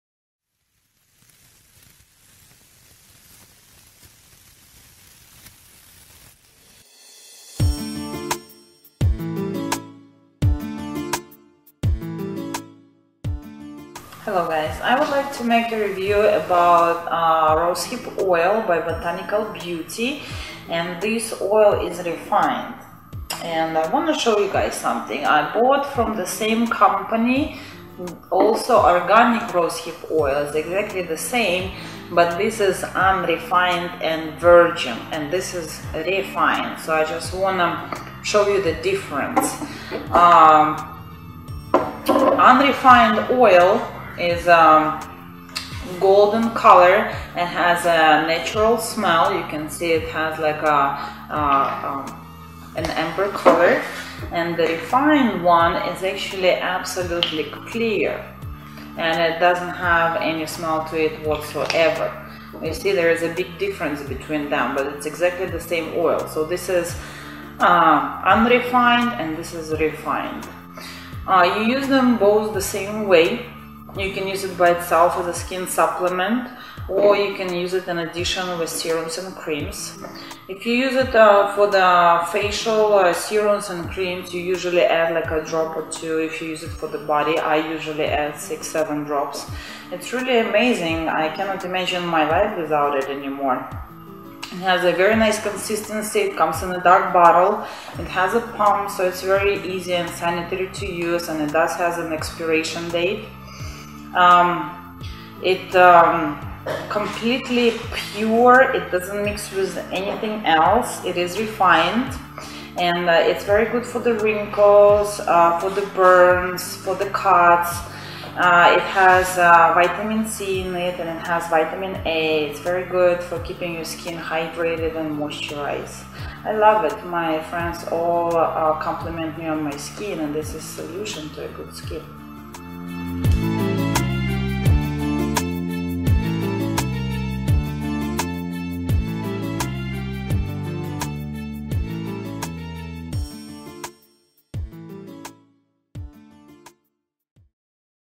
Difference between UNREFINED and REFINED Rosehip Oil. Did you know that? by Botanical Beauty
UNREFINED refers to oil being unprocessed, or untreated in any way. This oil has been cold pressed - this is done by filtering the seeds with air pressure, to remove any impurities, then milling the seeds into a paste. Pressure is then applied to force the oils to separate from the paste.

REFINED oil undergoes the process of cold pressing, but the process is then taken several more steps. After being pressed, some oils are then heated or subjected to cold temperatures. Often they are solvent treated to remove the scent or to make them a more desirable color.